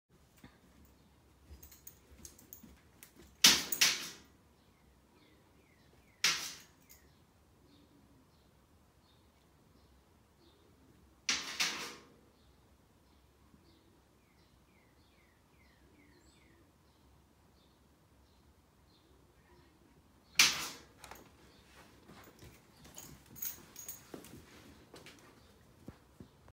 El impaciente chucho demanda salir a la calle golpeando un cubo de basura.
Poven, el caniche de 16 años, exige salir golpeando repetidamente el bote de basura de la cocina. ¡Mensaje recibido!